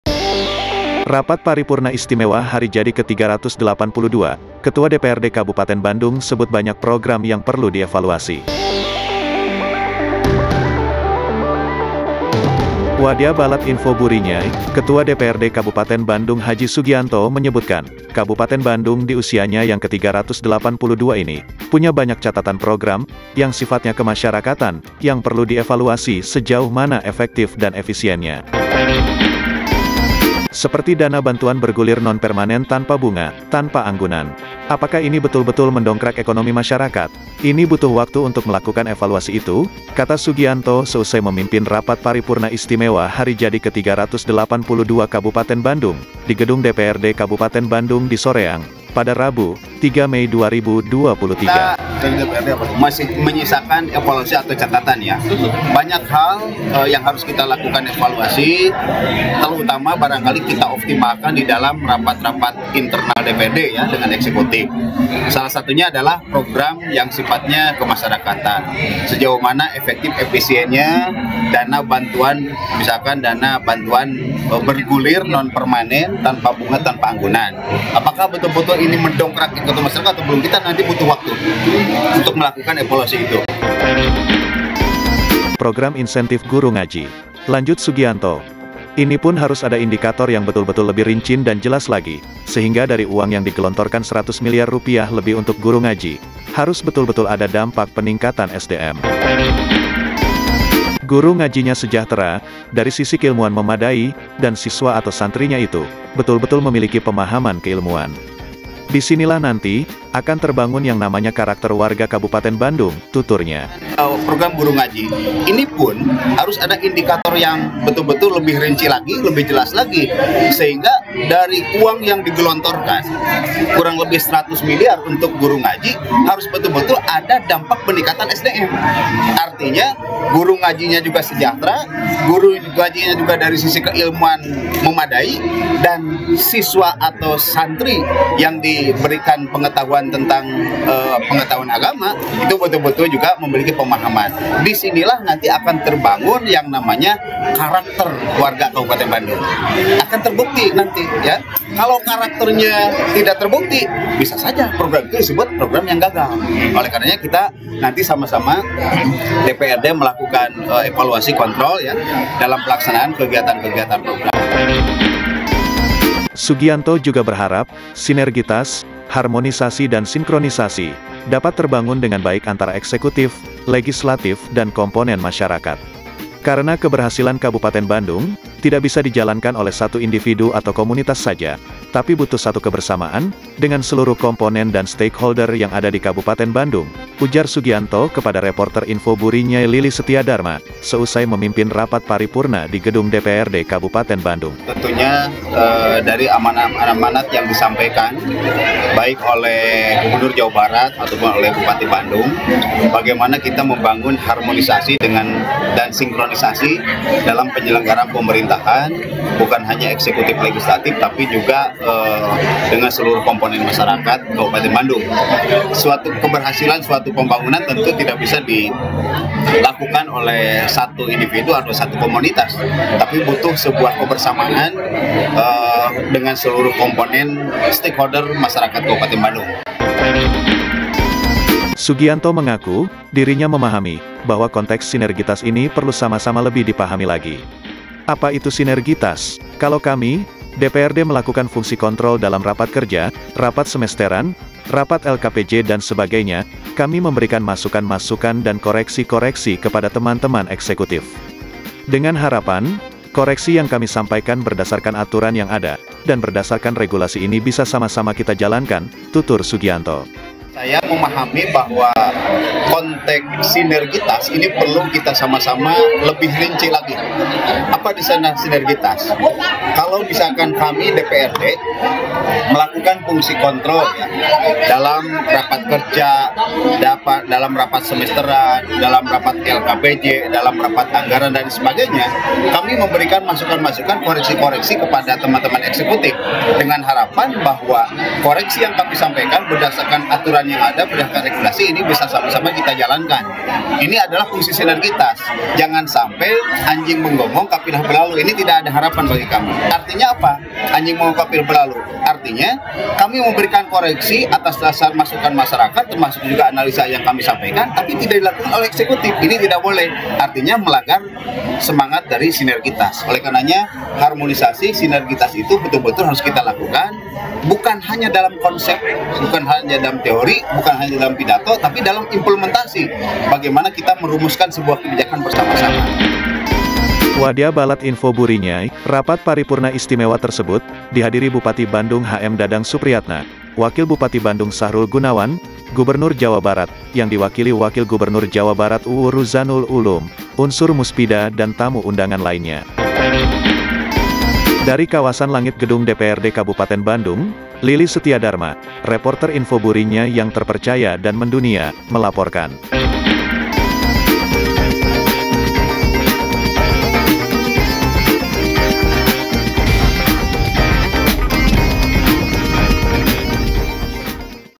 Rapat Paripurna Istimewa Hari Jadi Ke 382, Banyak Program yang Perlu Dievaluasi
SOREANG, INFO BURINYAY  – Ketua DPRD Kabupaten Bandung H. Sugianto menyebutkan, Kabupaten Bandung diusianya yang ke-382 ini punya banyak catatan program yang sifatnya kemasyarakatan yang perlu dievaluasi sejauh mana efektif dan efisiennya.

“Seperti dana bantuan bergulir nonpermanen tanpa bunga, tanpa anggunan. Apakah ini betul-betul mendongkrak ekonomi masyarakat? Ini butuh waktu untuk melakukan evakuasi itu,” kata Sugianto seusai memimpin Rapat Paripurna Istimewa Hari Jadi Ke-382 Kabupaten Bandung di Gedung DPRD Kabupaten Bandung di Soreang, Rabu (3/5/2023).

Yang kedua, lanjut Sugianto, program insentif guru ngaji. “Inipun harus ada indikator yang betul-betul lebih rinci dan jelas lagi sehingga dari uang yang digelontorkan Rp 100 miliar lebih untuk guru ngaji harus betul-betul ada dampak peningkatan SDM. Guru ngajinya sejahtera, dari sisi keilmuan memadai, dan siswa atau santrinya itu betul-betul memiliki pemahaman keilmuan. Di sinilah nanti akan terbangun yang namanya karakter warga Kabupaten Bandung,” tuturnya.

Sugianto juga berharap, sinergitas, harmonisasi dan sinkronisasi dapat terbangun dengan baik antara eksekutif, legislatif dan komponen masyarakat.

“Karena keberhasilan Kabupaten Bandung tidak bisa dijalankan oleh satu individu atau komunitas saja, tapi butuh satu kebersamaan dengan seluruh komponen dan stakeholder yang ada di Kabupaten Bandung,” ujar Sugianto kepada Reporter Info Burinyay seusai memimpin Rapat Paripurna di Gedung DPRD Kabupaten Bandung

Sugianto mengaku, dirinya memahami bahwa konteks sinergitas ini perlu sama-sama lebih dipahami lagi.

“Apa itu sinergitas? Kalau kami DPRD melakukan fungsi kontrol dalam rapat kerja, rapat semesteran, dan rapat LKPJ, dan sebagainya, kami memberikan masukan-masukan dan koreksi-koreksi kepada teman-teman eksekutif. Dengan harapan koreksi yang kami sampaikan berdasarkan aturan yang ada dan berdasarkan regulasi ini bisa sama-sama kita jalankan,” tutur Sugianto.

Rapat Paripurna istimewa tersebut dihadiri Bupati Bandung HM. Dadang Supriatna, Wakil Bupati Bandung Sahrul Gunawan, Gubernur Jawa Barat yang diwakili Wakil Gubernur Jawa Barat Uu Ruzhanul Ulum, unsur Muspida dan tamu undangan lainnya.

Liputan Reporter Info Burinyay : LILY SETIADARMA